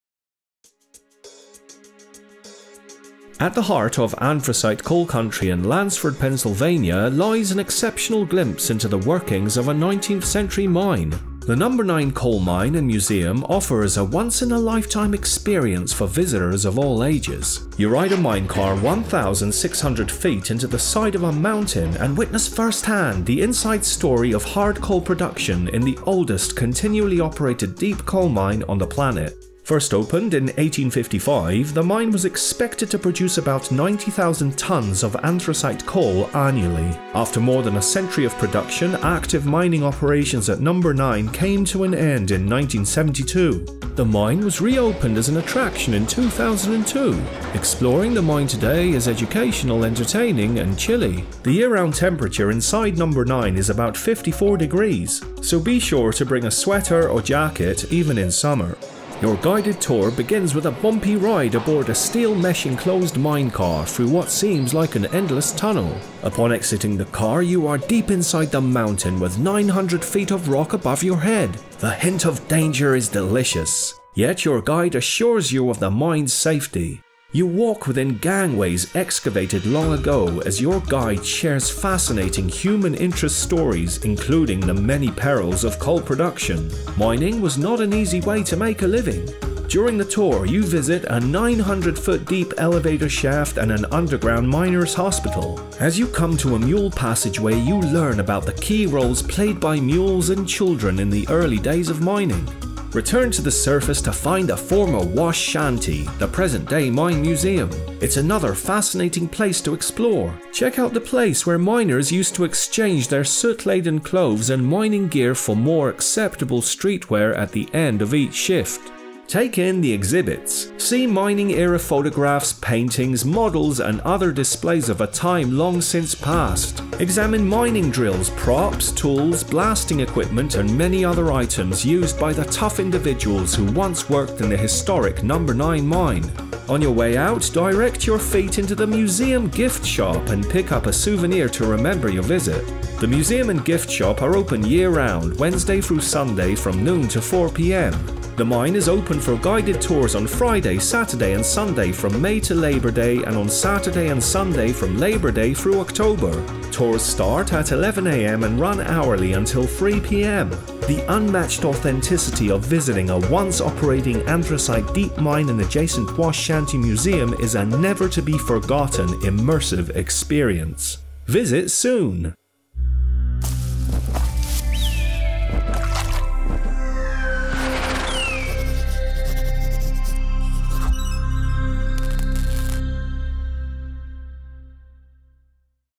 Number 9 Coal Mine
At the heart of anthracite coal country in Lansford Pennsylvania, lies an exceptional glimpse into the workings of a nineteenth century mine.  Visit the Number 9 Coal Mine and Museum.